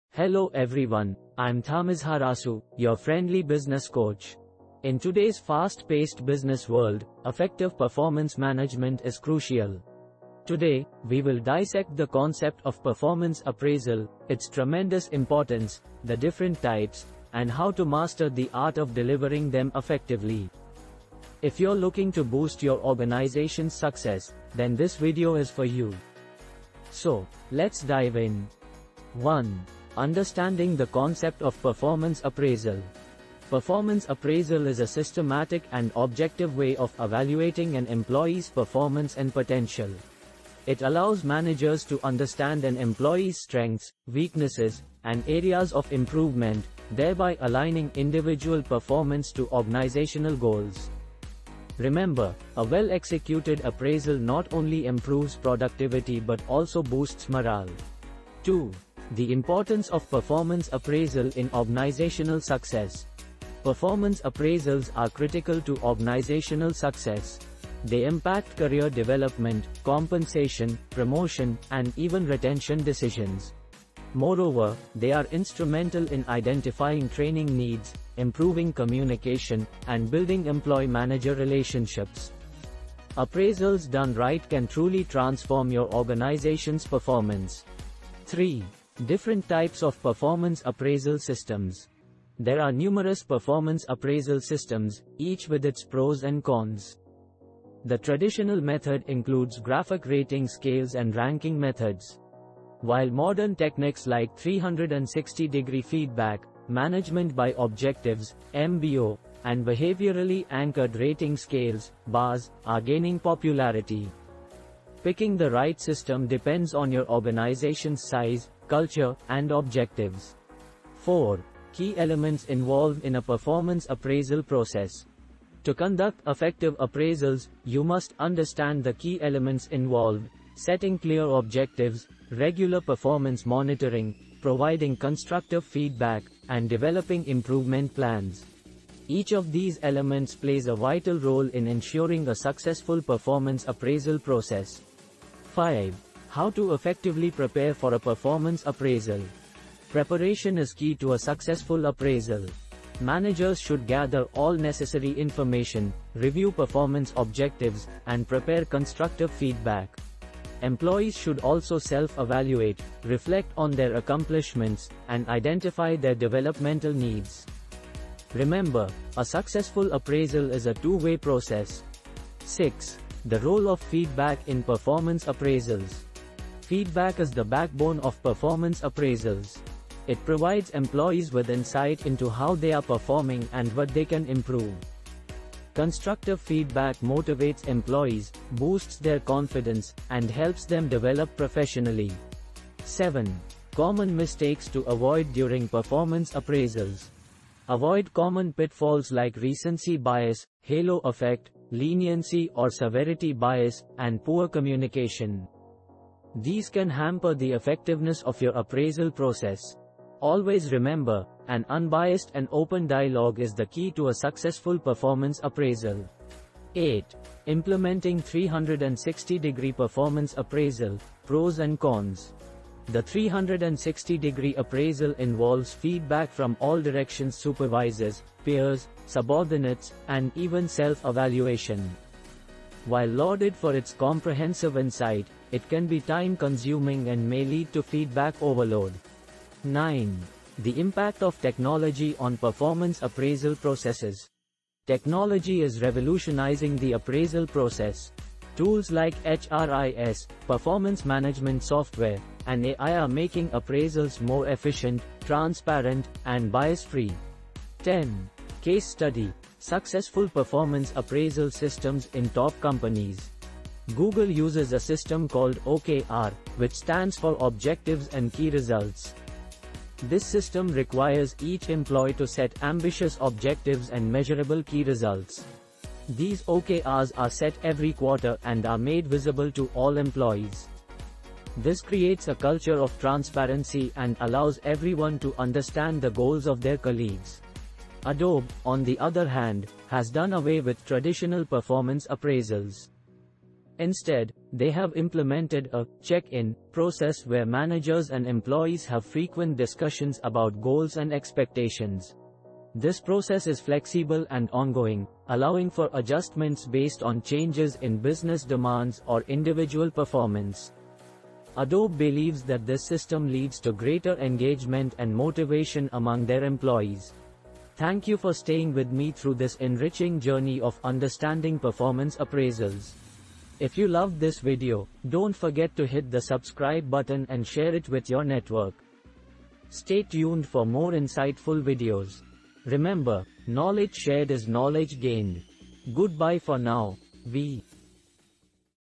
performance appraisal | Mastering the Art of Effective Performance Appraisal: A Guide for Managers
Hello everyone, I’m Thamizharasu, your friendly Business Coach. In today's fast-paced business world, effective performance management is crucial.
Today, we will dissect the concept of performance appraisal, its tremendous importance, the different types, and how to master the art of delivering them effectively. If you're looking to boost your organization's success, then this video is for you. So, let's dive in!

1. Understanding the Concept of Performance Appraisal
Performance Appraisal is a systematic and objective way of evaluating an employee's performance and potential.
It allows managers to understand an employee's strengths, weaknesses, and areas of improvement, thereby aligning individual performance to organizational goals. Remember, a well-executed appraisal not only improves productivity but also boosts morale.

2. The Importance of Performance Appraisal in Organizational Success
Performance appraisals are critical to organizational success. They impact career development, compensation, promotion, and even retention decisions.
Moreover, they are instrumental in identifying training needs, improving communication, and building employee-manager relationships. Appraisals done right can truly transform your organization's performance.

3. Different Types of Performance Appraisal Systems
There are numerous performance appraisal systems, each with its pros and cons. The traditional method includes Graphic rating scales and Ranking methods,
 while modern techniques like 360-degree feedback, Management by Objectives (MBO), and Behaviorally Anchored Rating Scales (BARS) are gaining popularity. Picking the right system depends on your organization's size, culture, and objectives.

4. Key Elements Involved in a Performance Appraisal Process
To conduct effective appraisals, you must understand the key elements involved – Setting clear objectives, regular performance monitoring, providing constructive feedback, and developing improvement plans.
 Each of these elements plays a vital role in ensuring a successful performance appraisal process.

5. How to Effectively Prepare for a Performance Appraisal
Preparation is key to a successful appraisal. Managers should gather all necessary information, review performance objectives, and prepare constructive feedback.
Employees should also self-evaluate, reflect on their accomplishments, and identify their developmental needs. Remember, a successful appraisal is a two-way process.

6. The Role of Feedback in Performance Appraisals
Feedback is the backbone of performance appraisals. It provides employees with insight into how they are performing and what they can improve. Constructive feedback motivates employees, boosts their confidence, and helps them develop professionally.

7. Common Mistakes to Avoid During Performance Appraisals
Avoid common pitfalls like recency bias, halo effect, leniency or severity bias, and poor communication. These can hamper the effectiveness of your appraisal process. Always remember, an unbiased and open dialogue is the key to a successful performance appraisal.

8. Implementing 360-Degree Performance Appraisal: Pros and Cons
The 360-degree appraisal involves feedback from all directions - supervisors, peers, subordinates, and even self-evaluation. While lauded for its comprehensive insight, it can be time-consuming and may lead to feedback overload.

9. The Impact of Technology on Performance Appraisal Processes
Technology is revolutionizing the appraisal process. Tools like HRIS, performance management software, and AI are making appraisals more efficient, transparent, and bias-free.

10. Case Study: Successful Performance Appraisal Systems in Top Companies
Google uses a system called OKR, which stands for Objectives and Key Results. This system requires each employee to set ambitious objectives and measurable key results.
These OKRs are set every quarter and are made visible to all employees. This creates a culture of transparency and allows everyone to understand the goals of their colleagues.

Adobe, on the other hand, has done away with traditional performance appraisals. Instead, they have implemented a 'check-in' process where managers and employees have frequent discussions about goals and expectations.

This process is flexible and ongoing, allowing for adjustments based on changes in business demands or individual performance. Adobe believes that this system leads to greater engagement and motivation among their employees.

How I help your business Growth?